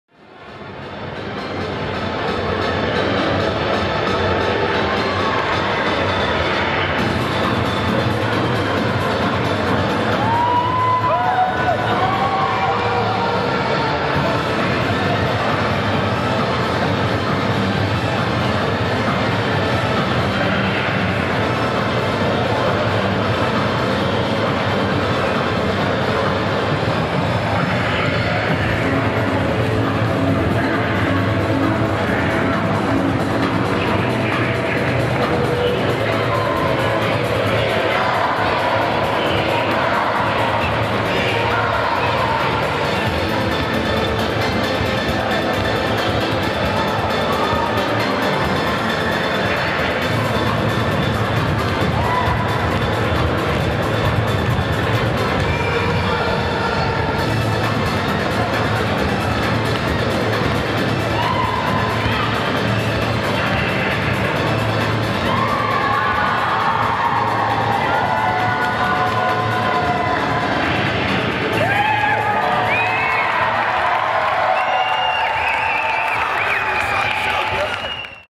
Your Son Is So Good!
Indulge a little "home movie."  This insane halftime basketball chaos is led by my son, Noah (green shirt).  Dances move names include, "Walking down to the court,"  "Pass the Ball to Me," "Free Throw," "Grab Snacks" and "Hold the Trophy Aloft."  Congrats to the ICSB Basketball teams! Please rate, comment, favorite and link! ;)